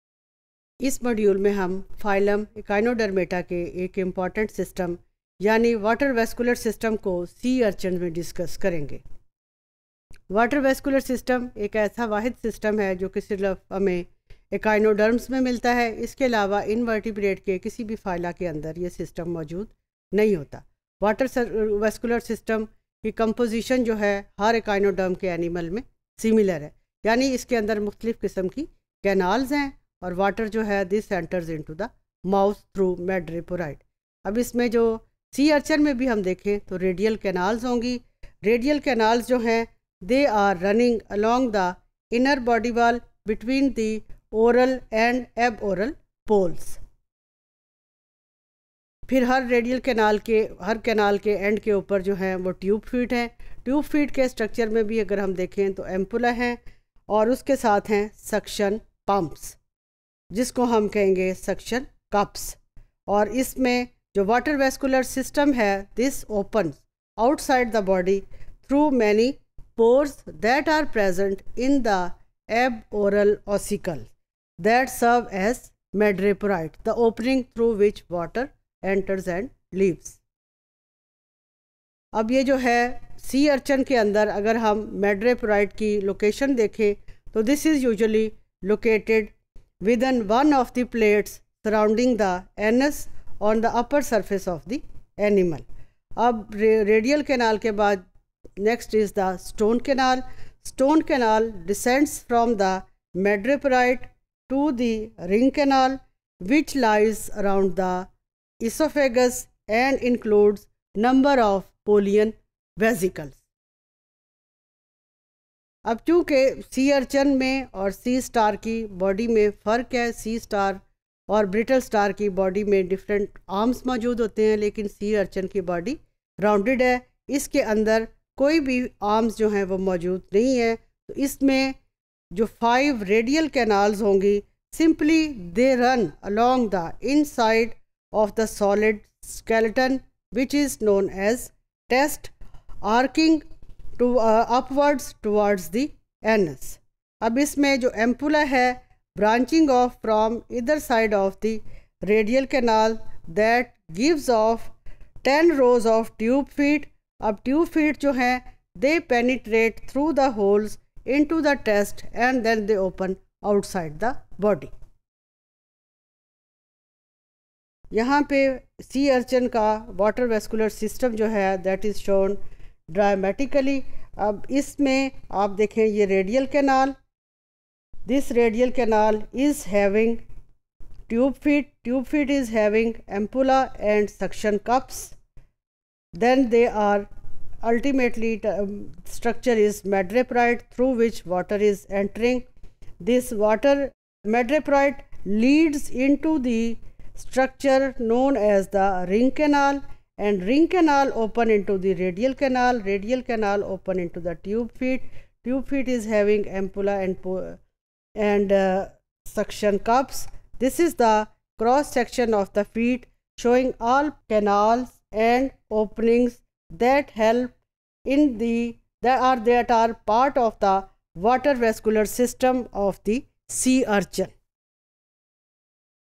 Water vascular system of sea urchins | Animal Diversity Invertebrates | ZOO512T_Topic247
Topic247: Water vascular system of sea urchins
by Dr. Farkhanda Manzoor